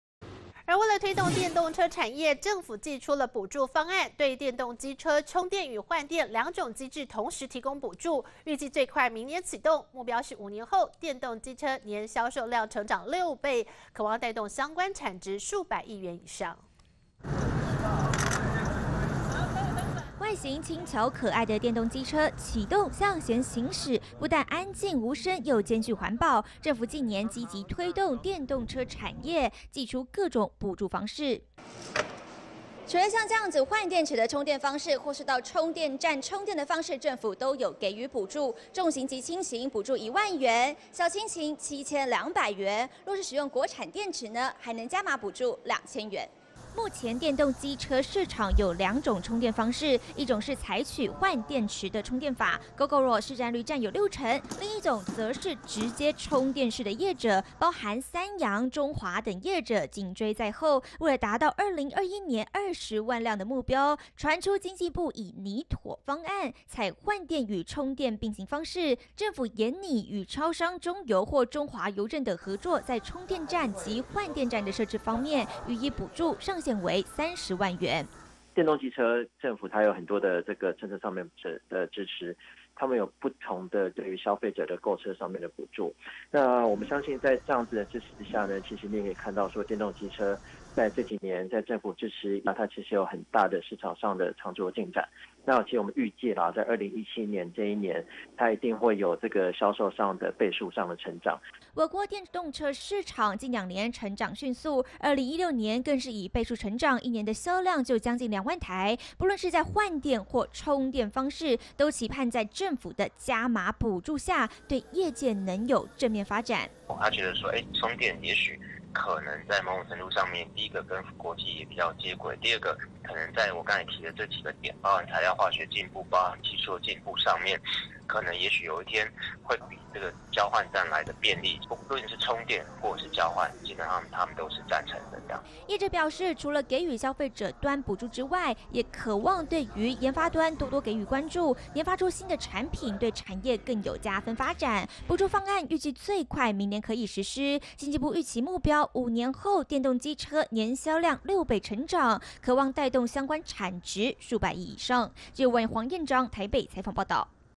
電動機車補助大加碼 Gogoro 特斯拉 等電動車貨物稅優惠至2021年 e-Moving推1.2萬有找
經部補助電動機車大加碼 Gogoro、特斯拉等電動車貨物稅優惠至2021年 e-Moving推1.2萬有找優惠方案 政府研擬電動機車產業推動方案，為兼顧多元化與國產化，電動機車將採換電與充電並行；充、換電站營運模式是否具競爭力將納補助考量；若電動車採用國產電池者，購車補助還可再加碼2,000元。

經濟部的補助是重型及輕型都是1萬元，小輕型是7,200元，如果是使用國產電池者，還可以再加2,000元。環保署及地方政府也另有加碼補助。

知情官員表示，電動機車產業推動方案，原本規劃以換電方式為主，以符合國際發展趨勢，國際上電池多採換電模式，但引來採充電方式廠商的不滿，經濟部與業界溝通後，目前將採換電與充電並行方式。官員說，兩者並行，「兄弟爬山各自努力」，由市場作決定，政府也會滾動檢討。

經濟部之前已向行政院長林全報告過電動機車產業推動策略方案，因目標設定過於保守，被要求重新調整，經濟部最近已擬妥方案，準備向林全報告後即可拍板，全案預計從明年起推動，期程五年。

政策目標是五年後電動機車年銷售量六倍成長，可望帶動相關產值數百億元以上；但當市場規模有單一車廠年銷售10萬輛以上，就回歸市場機制。

電動機車產業推動五年計畫，目標設定第五年開始每年銷售量達20萬輛，今年可望達三、四萬輛，等於成長六倍。官員表示，電池也要推統一規格，經濟部已請車輛相關公會推動，為確保規格化能落實，最快明年開始，考慮須符合規格化者，才能享有補助。

目前市面上最常見的電動車款包括電動機車 Gogoro 與電動小客車特斯拉，完稅價若在 140 萬元以下者，可依財政部規定先報繳貨物稅，等到完成牌照登記後再申請退還貨物稅款，形同免徵；超過 140 萬元者，則以超出部分減半課徵。不過財政部表示，該項貨物稅優惠僅至 2021 年年底，消費者若想購入電動車款，可得留意時限。

財政部北區國稅局表示，為響應綠色環保，達成節能減碳目標，並提高民眾購買電動車之意願，自今（ 2017 ）年 1 月至 2021 年 12 月 31 日止，若購買完全以電能為動力之電動車輛並完成新領牌照登記者，完稅價在 140 萬元以下者可依規定先報繳貨物稅，等到完成牌照登記後再申請退還貨物稅款，形同免徵，至於超過 140 萬元者，則以超出部分按規定稅率減半課徵。

若以「 Gogoro 2 」為例，含稅價為 7.38 萬元，因低於 140 萬元以下，自然可依規定先報繳貨物稅，等到完成牌照登記後再申請退還貨物稅款，形同免徵，且消費者購車還可享經濟部工業局補助電動車 1 萬元、環保署補助 4 千元、電動機車鋰電池補助 4 千元與地方縣市環保局 1 千元至 1.7 萬元補助，若為舊機車汰換新車，總計最多可享 3.5 萬元補助，等於實質價格僅 3.88 萬元。

不過，特斯拉由於完稅價格較高、補助也相當少，因此多以完稅價為實質購買價格。例如 6 月中交車的「 Model X 」最低價的 75D 系列基本款完稅價為 383 萬元，在貨物稅部分就得多繳納超出 140 萬元的部分乘以稅率 30% 再減半課徵，因此總計需繳納 36.45 萬元貨物稅。不過，若是在 2022 年才購買相同價格的特斯拉，屆時貨物稅就無 140 萬元的免徵額，得繳 57.15 萬元貨物稅，相差 20.7 萬元。

中華車推動電動機車雙管齊下，也推出車電分離方式，電池用租的，最低只要11,800元就可以騎回家，要大舉搶攻暑假市場。

中華車表示，在政府大力推動電動機車及經濟部加碼補助等利多政策下，民眾對於購買電動機車的意願逐漸增高，截至目前為止e-moving中華電動車總銷售較2016年同期成長24%，銷售量逐年刷新。中華汽車電動二輪車趁勢推出11,800元騎回家「豪省」方案，以同級市場最低價搶攻暑假購車旺季大餅。

隨著消費者對於電動機車的使用接受度日漸增高，e-moving除了提供原來的車電一體的銷售模式外，新推出e-moving車電分離「豪省」方案，消費者只需購買e-moving空車，即可用每月299元租用電池，享有租用期間電池終身不限里程永久保固的好康。